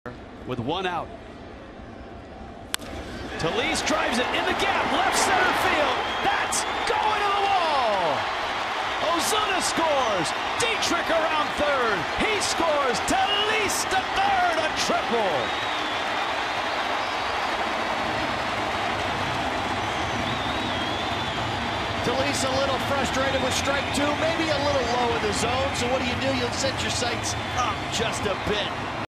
COL@MIA: Telis drives in two with a triple to left
Tomas Telis hits a triple in the left-center-field gap and drives in a pair of runs to give the Marlins a 6-3 lead in the bottom of the 8th